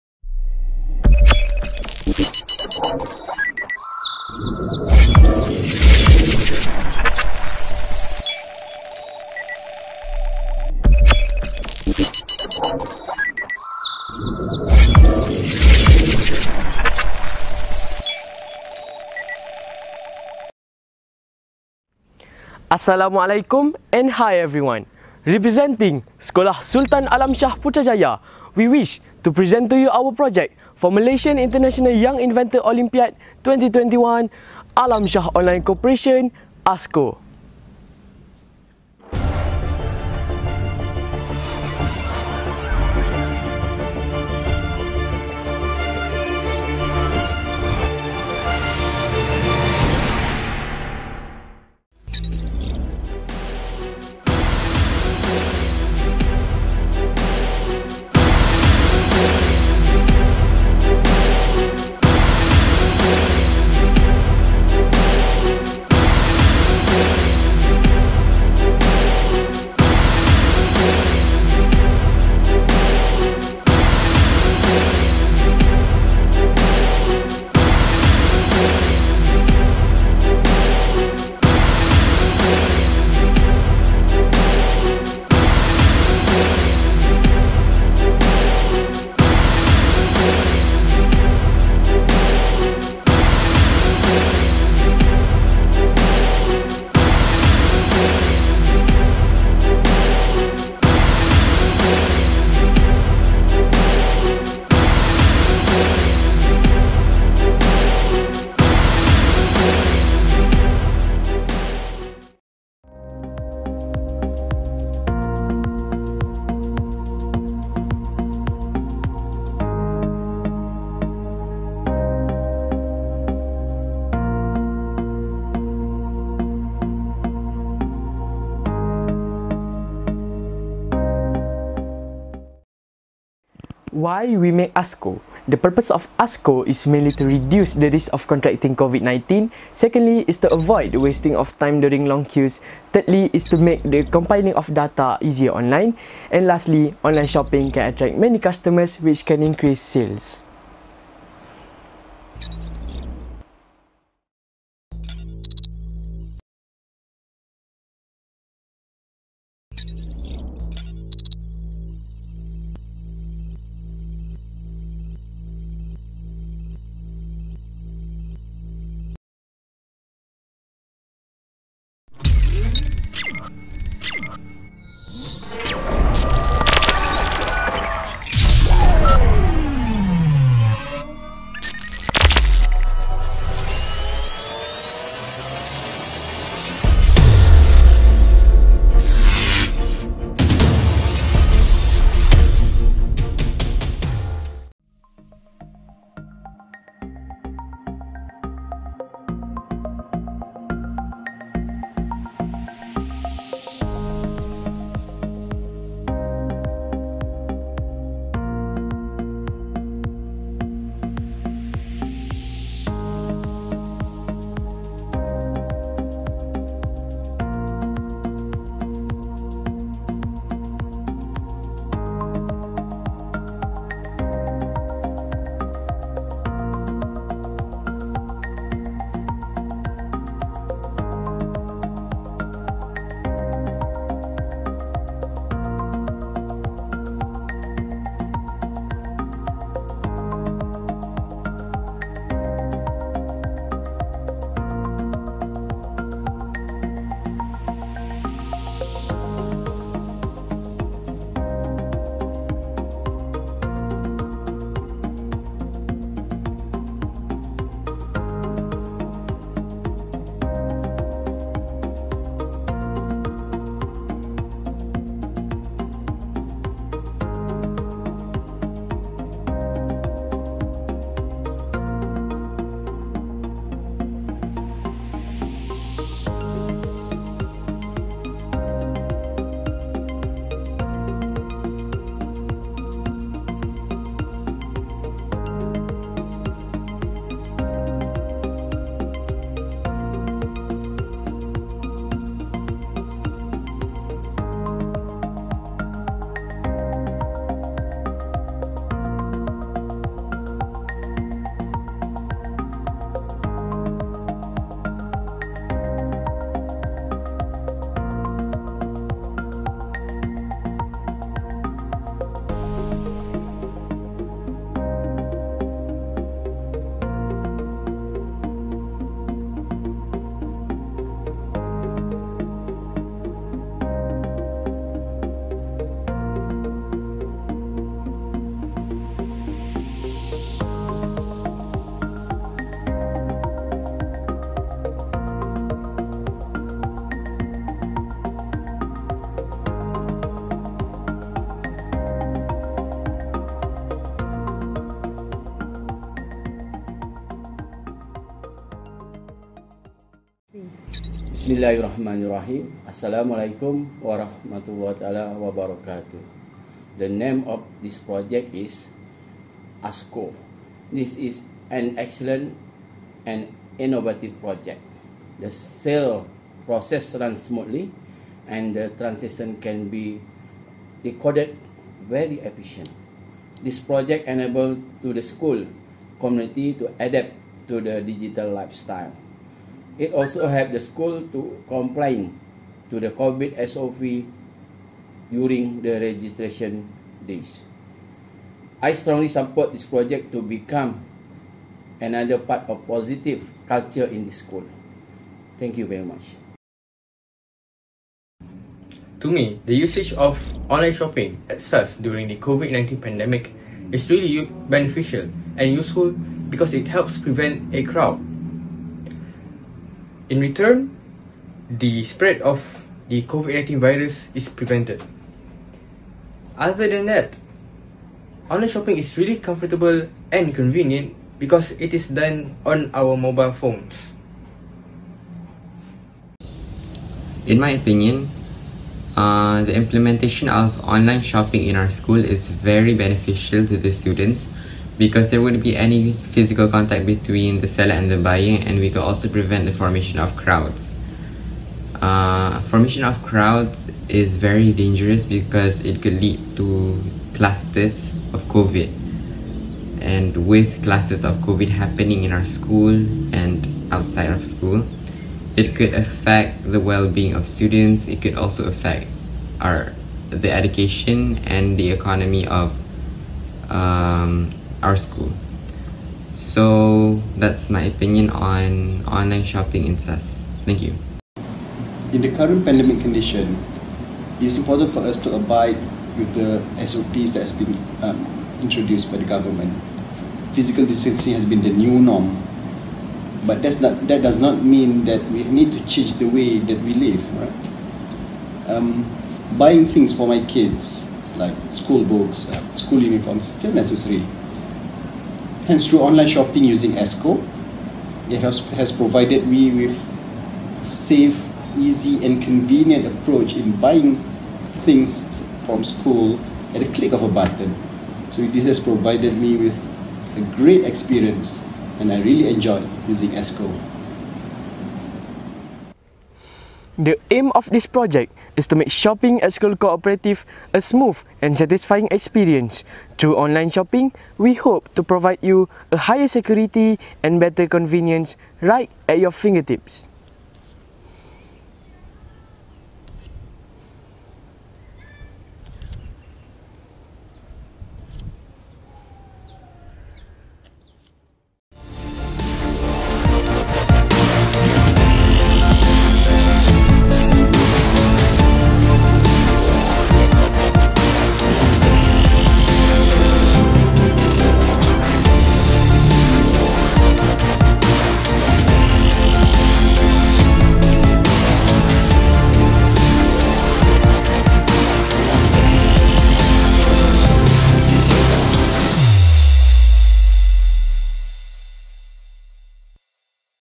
MIYIO 2021 SEKOLAH SULTAN ALAM SHAH PUTRAJAYA - ALAM SHAH ONLINE CORPORATION (ASCO)
Assalammualaikum and Hi everyone,

We are from Sekolah Sultan Alam Shah, Putrajaya presenting to you our project, Alam Shah Online Corporation (ASCO) for the submission of Malaysian International Young Inventors Olympiad (MIYIO) 2021.

We would like to appreciate all the hard work and diligent effort that we have put into completing this project. We are gratefully to have our supervisor, Miss Norsyidah binti Abdullah, team members, teachers, parents and friends for contributing in bringing this project to a great success.